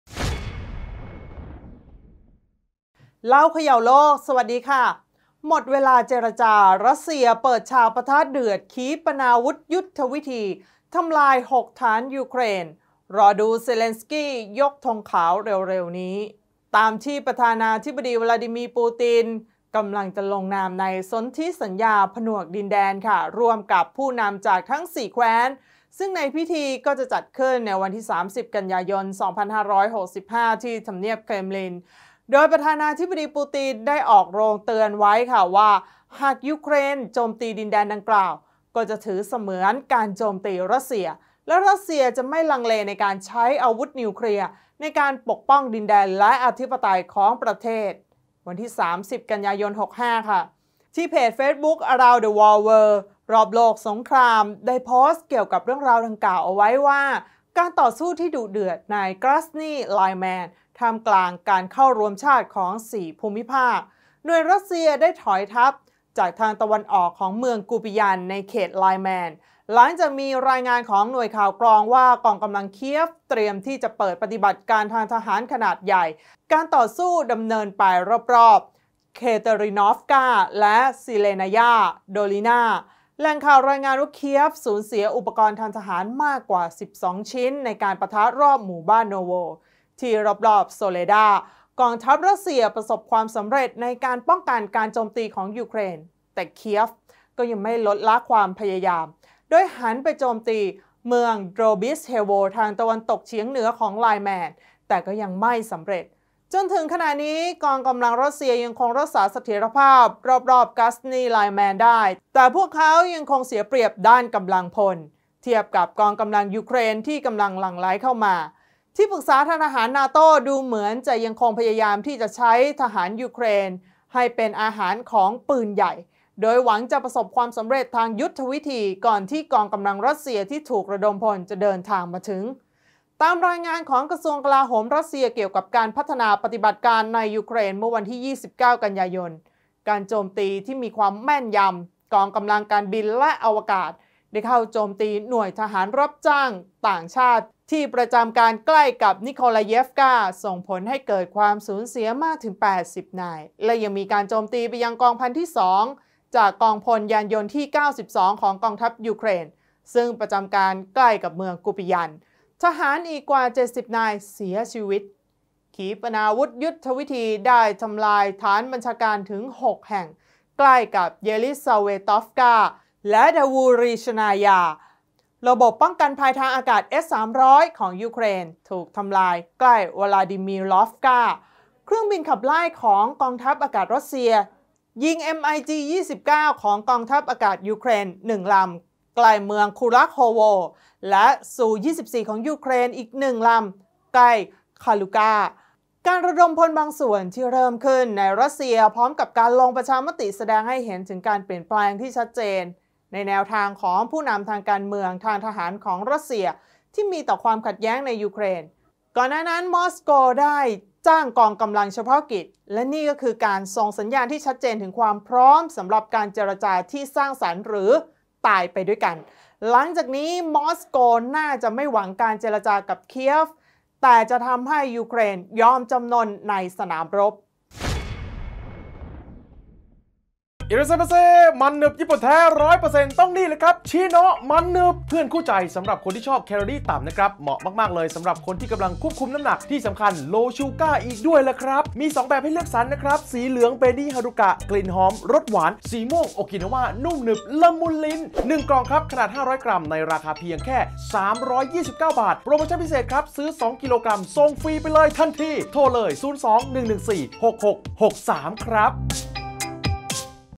หมดเวลาเจรจา รัสเซียเปิดฉากปะทะเดือด “ขีปนาวุธยุทธวิธี” ทำลาย 6 ฐานนยูเครน รอดูยูเครนยกธงขาวเร็วๆนี้
วลาดีมีร์ ปูติน